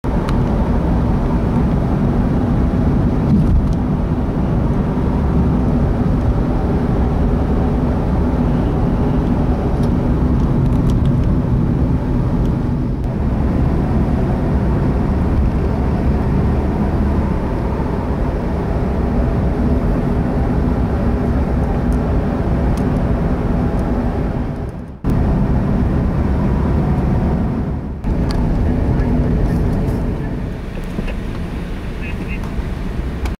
Charger R.C.M.P Police Car
Not your typical R.C.M.P. vehicle around Moncton, usually Police drive Crown Victoria's. A Charger cruising on Mountain Road this afternoon. Looks like Ford might be getting competition!
link to live Police EMS scanner feed